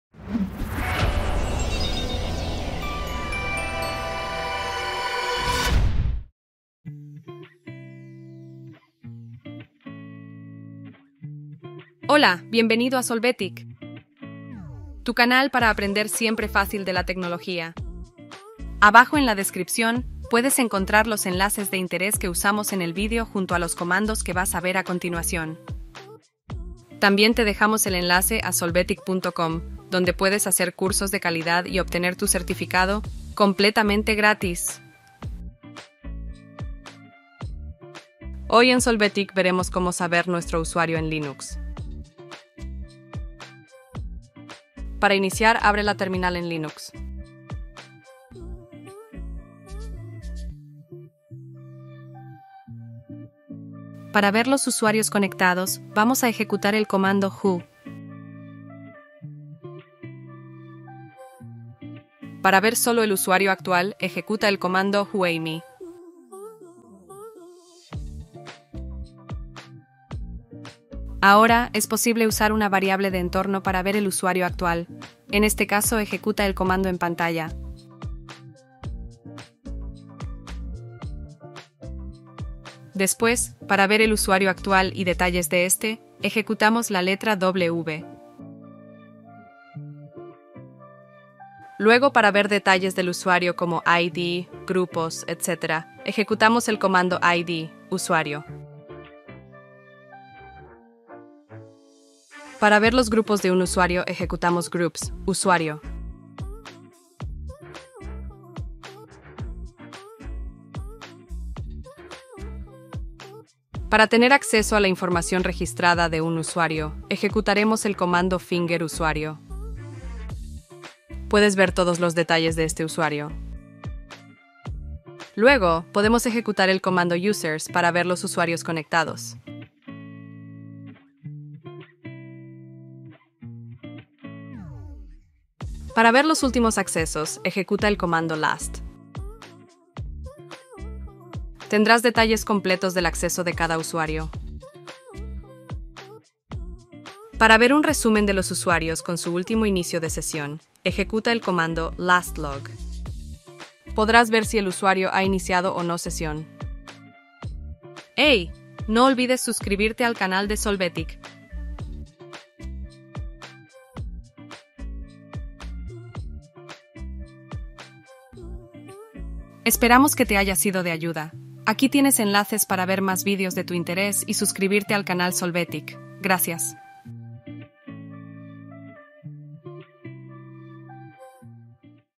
Cómo Saber que Usuario soy en Linux
¿No sabes que usuario soy en Linux? No te preocupes, en este vídeo tienes los comandos necesarios para Saber que Usuario soy en Linux en pocos segundos y conocer más información sobre ello de forma completa.

🔹COMANDOS🔹
who
whoami
echo $USER
w
id “usuario”
groups “usuarios”
finger “usuario”
users
last
lastlog
who
whoami
echo $USER
w
id “user”